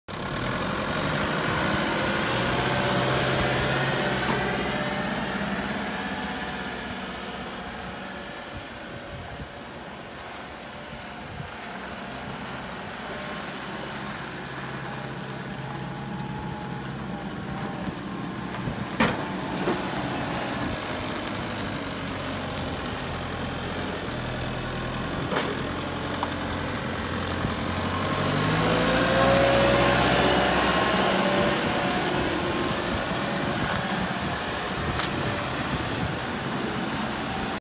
Köp Redskapsbärare Wille 455B på Klaravik
Redskapsbärare Wille 455B
Fabrikat: Wille
Modell: 455B
År: 2006

Mätarställning: Cirka 8000h (mätare trasig)
Drivmedel: Diesel
CE-märkt: Ja

Utrustad med:
- Centralsmörjning Ny 2015
- 3e och 4e funktion
- AC Fungerar
- Vikplog
- Sandspridare
- Motorhydraulik fram och bak